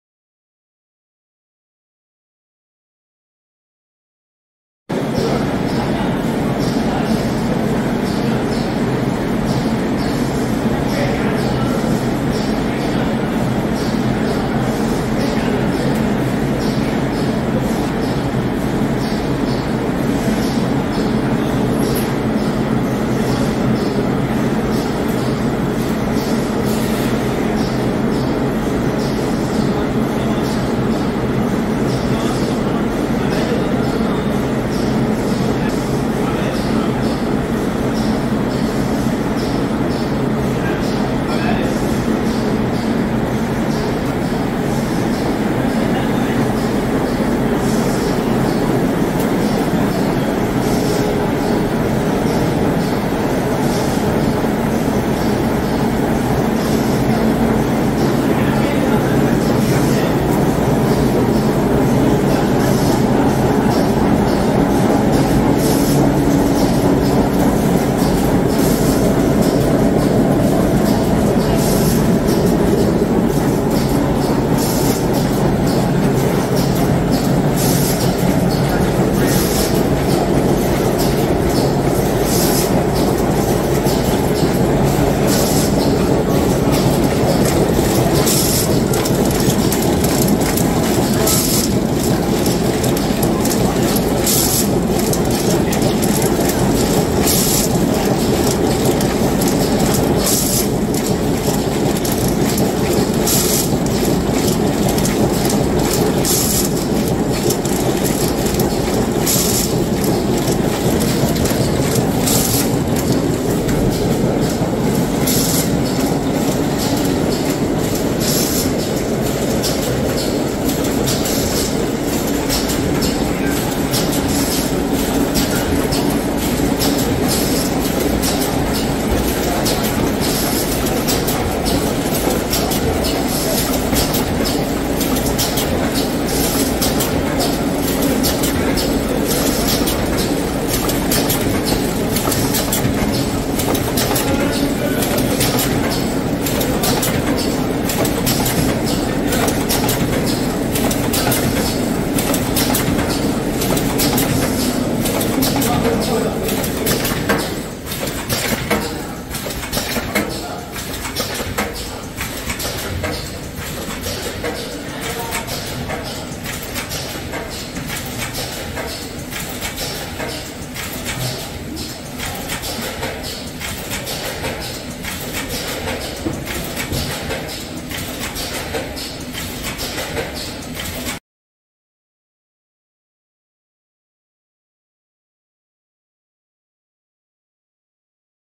Линия для производства влажных салфеток SYT20
Предлагаем вашему вниманию видео работы линии для производства влажных салфеток SYT20. С полным описанием и характеристиками вы можете ознакомиться, посетив наш сайт